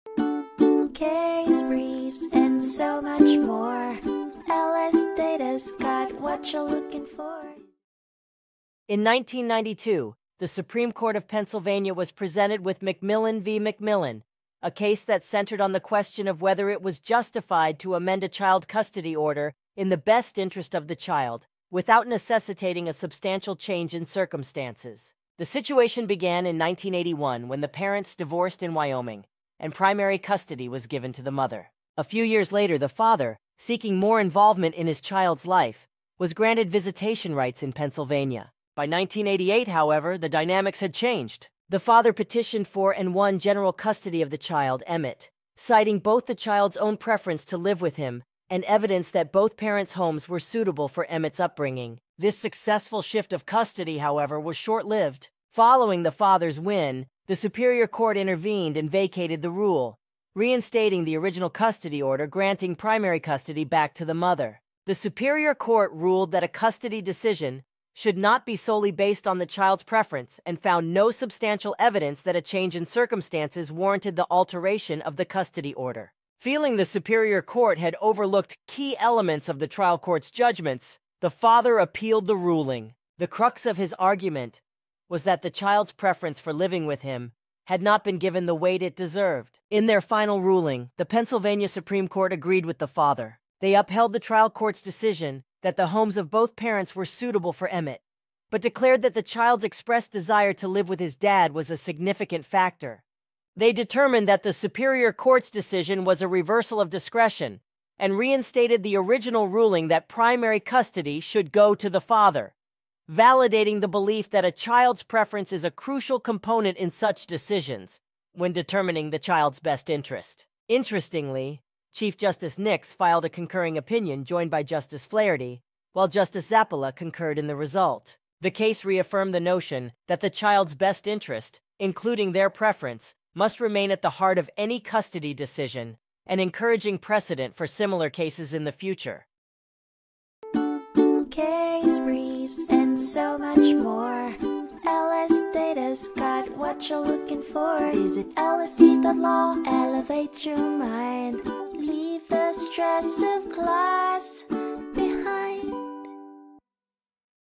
McMillen v. McMillen (1992) Overview | LSData Case Brief Video Summary
The father is appealing a decision by the Superior Court to reinstate a child custody order that granted primary custody to the mother. The parents were divorced in Wyoming in 1981, with primary custody given to the mother. The father sought partial custody in Pennsylvania, which was granted as visitation rights. The father won general custody in 1988, citing the son's preference and suitability of both homes. The Superior Court vacated the custody order and reinstated the previous order, stating that the child's best interests wouldn't be served by changing custody based solely on the child's wishes.

The most relevant facts to the court's analysis are the child's preference to live with his father, the suitability of both parents' homes, and the fact that the father doesn't need to demonstrate a substantial change in circumstances to modify the custody order.

McMillen v. McMillen (1992)
Supreme Court of Pennsylvania
529 Pa. 198, 602 A.2d 845